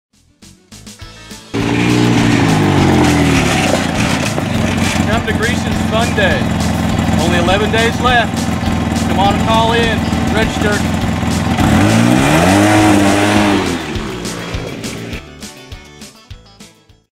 Greeson ORV Fun Day by Paul
Join us for our first ever ORV Fun Day at Hot Springs ORV Park September 15th from 10 am to 3 pm.  Trail rides, demo rides, catered lunch, poker and run and much more!  Hope to see you there!